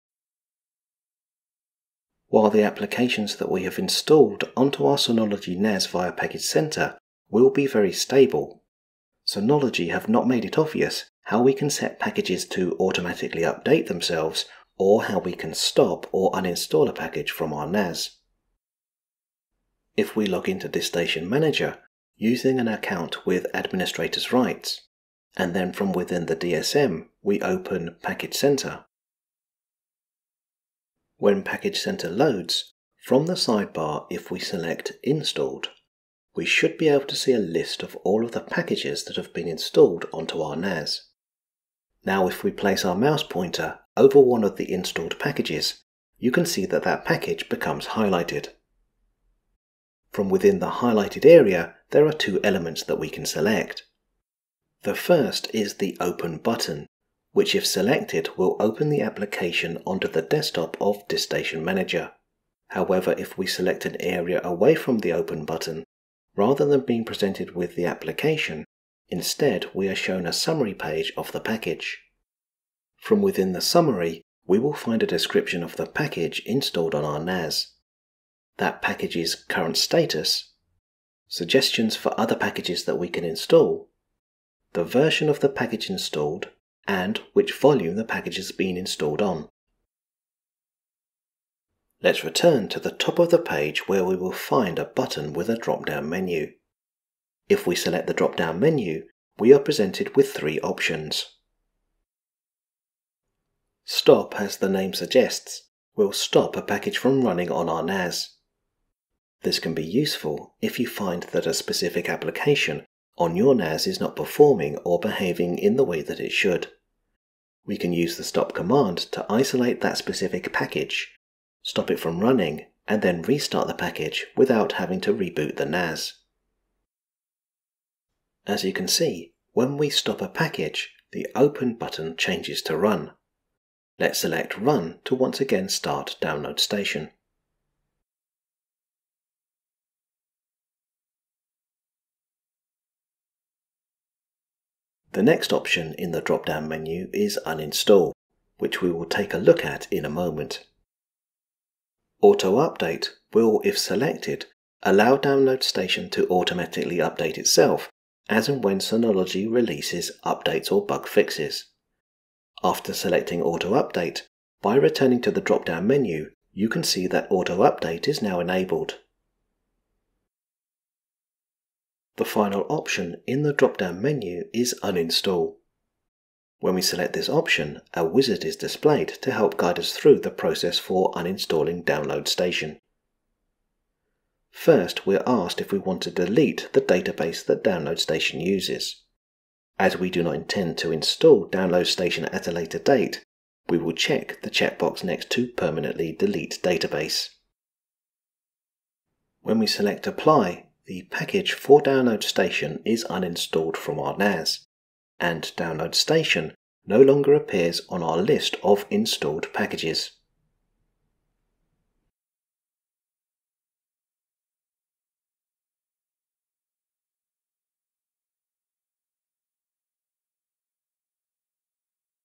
Synology NAS tip - How to Stop, Auto Update or Uninstall a Package on your NAS
In this video we look at how you can control the packages that have been installed on your Synology NAS. We will show you where to go if you need to control a package. Then we will review how you can either, Stop a package from running, set a package to auto update itself or if need be, uninstall a package.

Products seen in this video:

Synology DS218j 2 Bay Desktop NAS
Western Digital Red 2Gb HD
Seagate Iron Wolf 2Gb HD